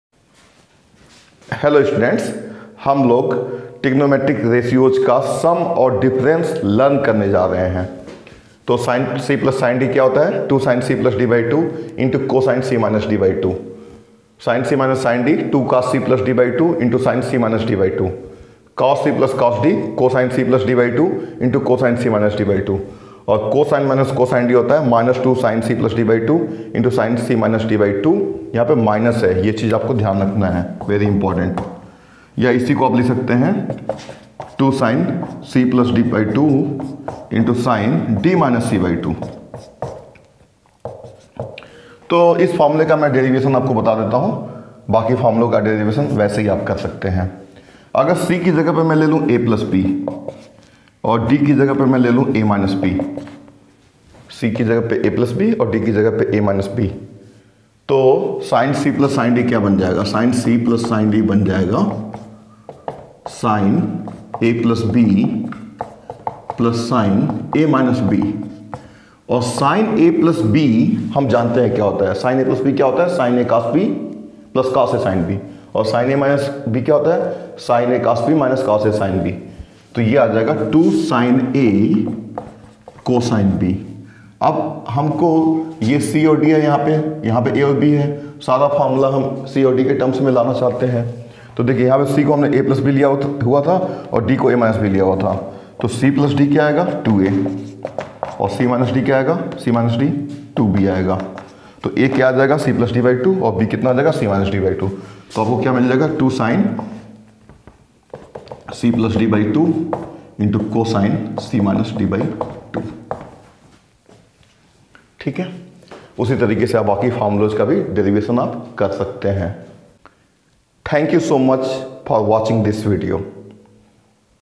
Sum and difference of trigonometric ratios|Class 11|Part 7
Hi Friends!

This  video deals with the Sum and difference formula of Trigonometric ratios

Category
Education